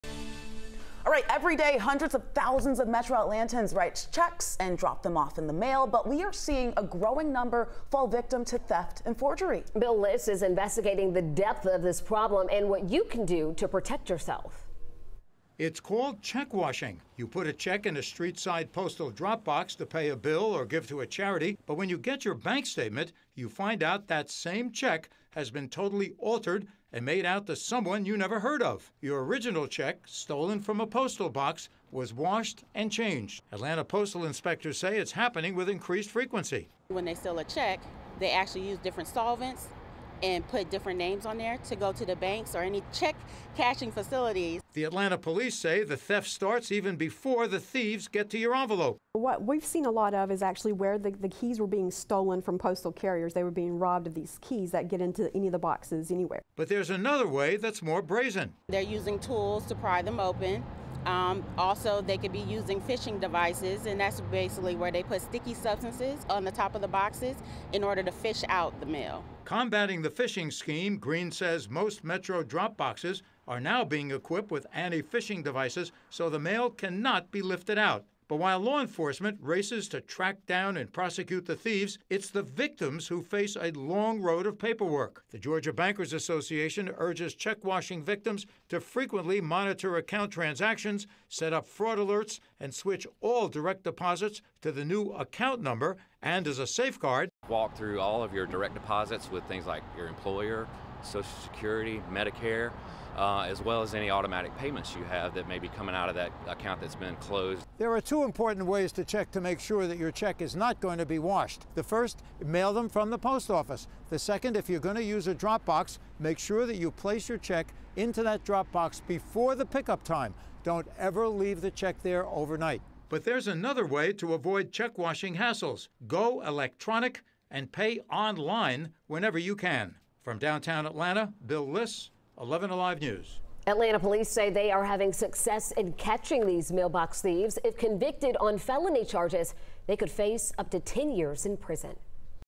How to protect yourself from check-washing schemes
Thieves are breaking into U.S. Postal Drop Boxes, stealing checks, then "washing" the contents and replacing with fake information, cashing them and making big money